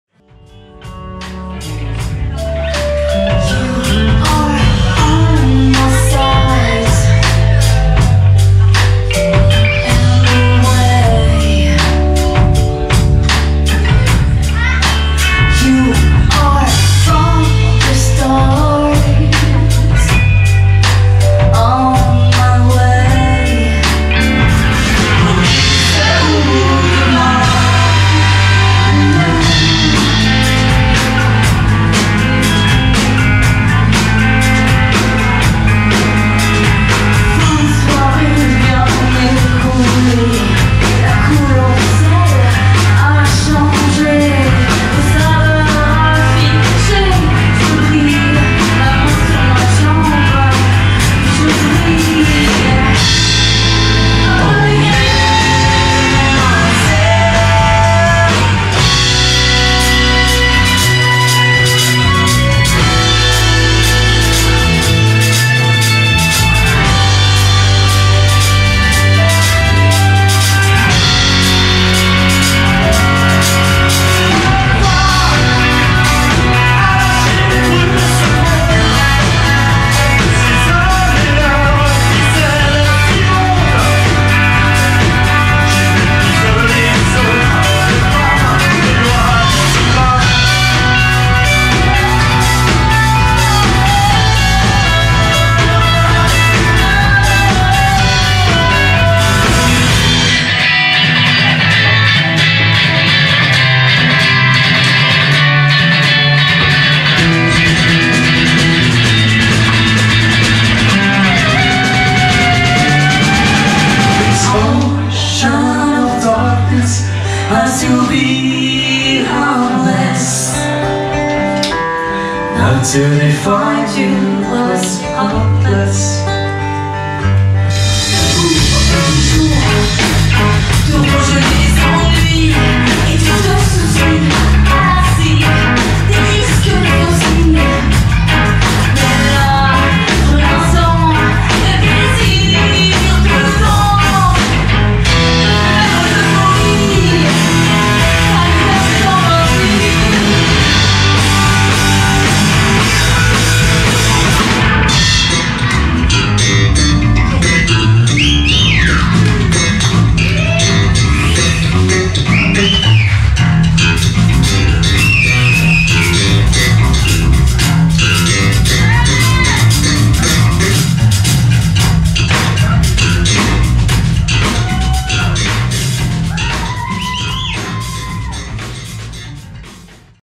Donkey Shots | Teaser - Live aux Ursulines, Crémieu 15/10/2022
| Singers / Chanteurs : Aurélie Kavafian, Hubert Blondet, Fabien Moreau
| Guitars / Guitares : David Delassus, Fabien Moreau, Hubert Blondet
| Keyboard / Clavier : Fabien Moreau
| Recorder / Flûte : Fabien Moreau
| Bass / Basse : Rudy Belloni
| Drum / Batterie : Alessandro Catarsi